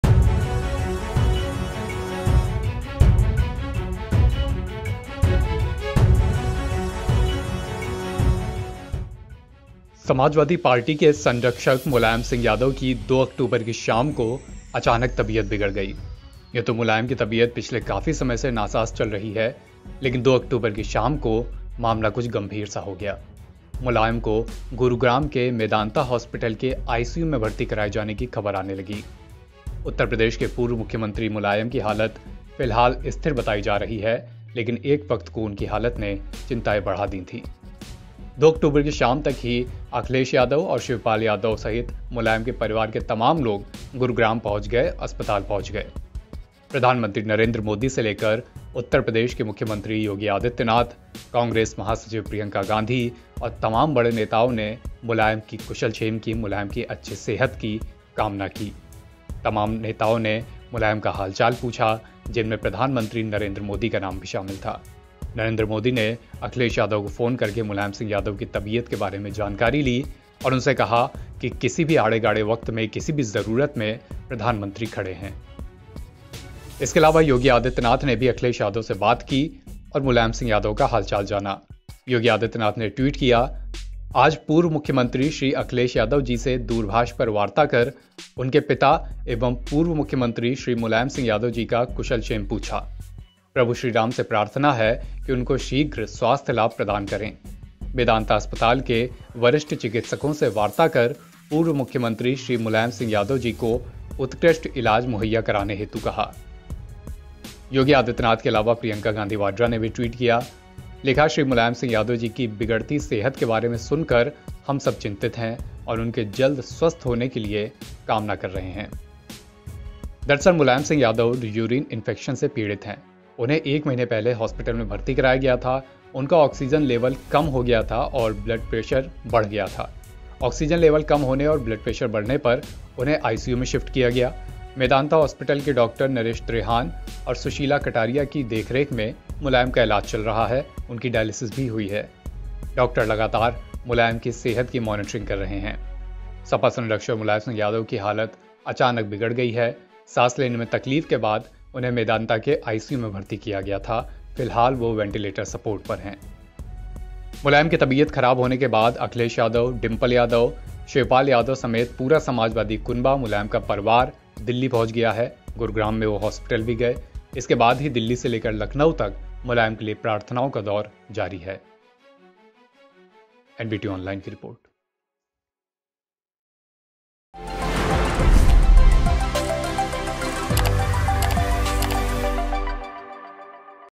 Mulayam Singh Yadav Health News : Akhilesh Yadav भागे Medanta । PM Modi, CM Yogi का फोन पहुंचा | NBT
Mulayam Singh Yadav Health News : Akhilesh Yadav भागे Medanta । PM Modi, CM Yogi का फोन पहुंचा

Mulayam Singh Yadav Health News : Akhilesh Yadav rushes to Medanta Gurugram PM Modi, CM Yogi made him a call. Mulayam Singh Yadav ki tabiyat kaisi hai asks PM Modi to Akhilesh Yadav. पीएम मोदी ने अखिलेश यादव को फोन करके मुलायम सिंह यादव का हाल जाना